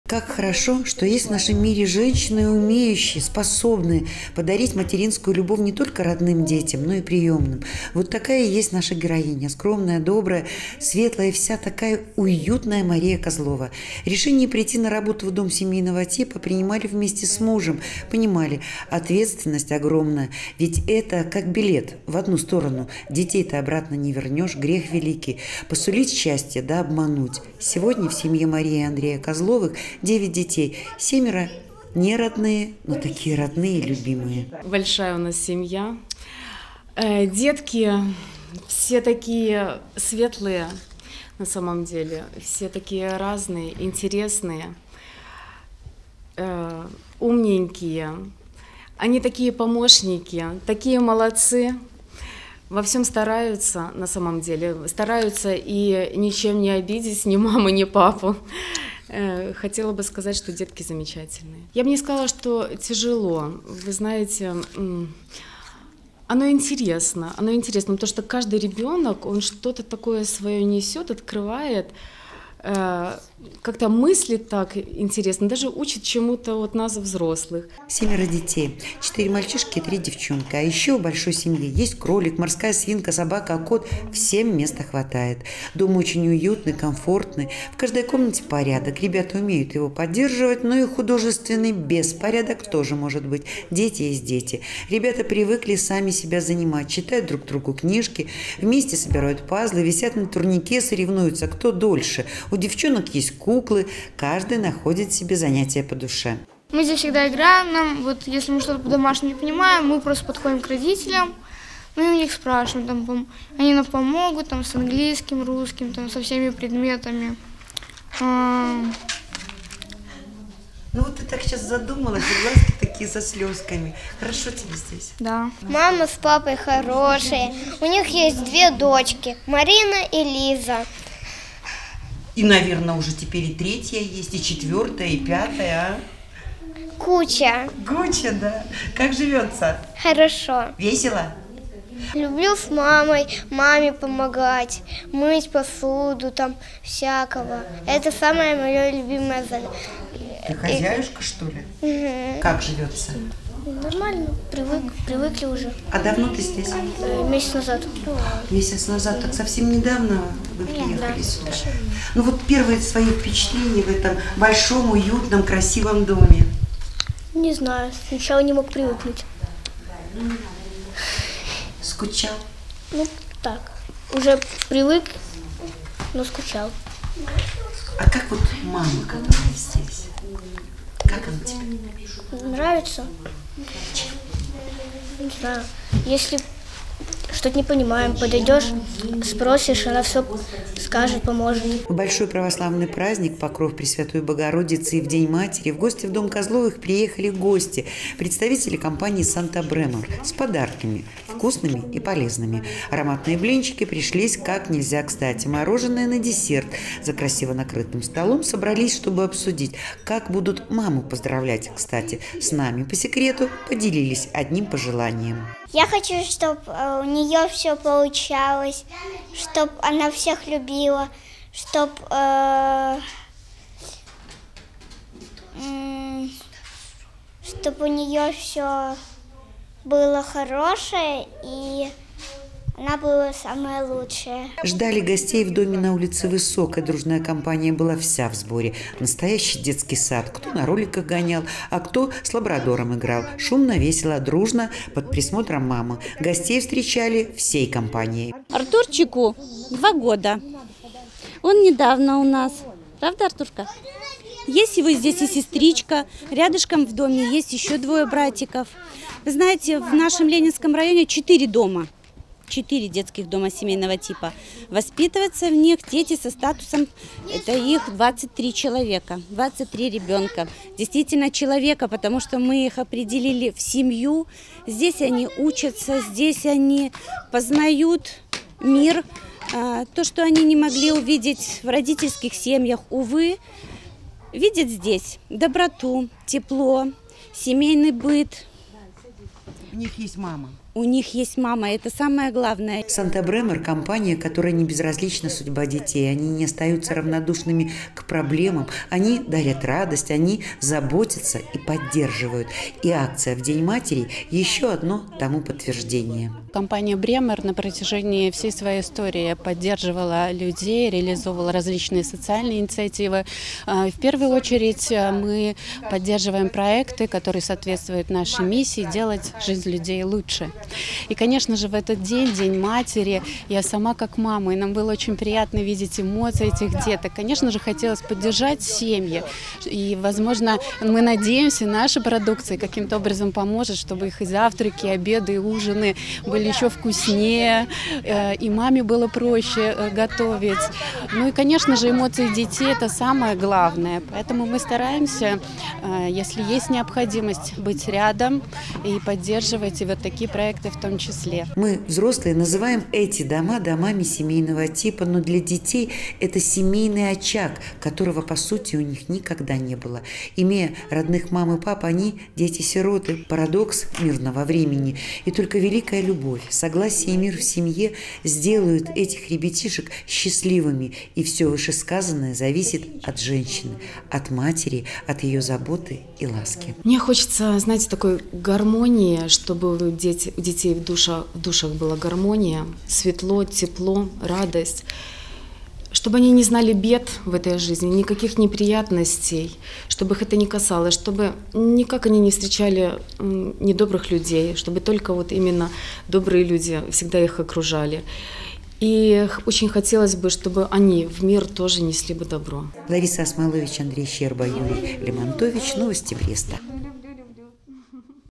2022-10-14 г. Брест. Поздравление мам из Домов семейного типа. Новости на Буг-ТВ.
Поздравление мам из Домов семейного типа от компании «Санта Бремор».
День матери - прекрасный повод собраться в семейном кругу, наполнить дом душевной теплотой, уделив все внимание маме. Мама - какое емкое красивое слово, испокон веков женщина окружена святым ореолом материнства. Со слова "МАМА" мы учимся говорить и осознавать своё предназначение в этом мире. В этом слове есть любовь и надежда, сила и вдохновение, в нем свет, тепло и уют. Безмерно счастлив тот человек, которого заботливые материнские руки и добрые слова поддерживают не только в детстве. К сожалению, не всегда женщины понимают свое истинное предназначение, они подменяют его другими понятиями, отказываясь от своих детей. Но эти ребята не остаются без внимания и заботы. Сегодня в Беларуси функционируют дома семейного типа, в Бресте в Ленинском районе таковых четыре. Сегодня к ним с поздравлениями и подарками приехали специалисты кампании «Санта Бремор».  О теплой встрече, об эмоциях детей и взрослых - расскажет Лариса Осмолович.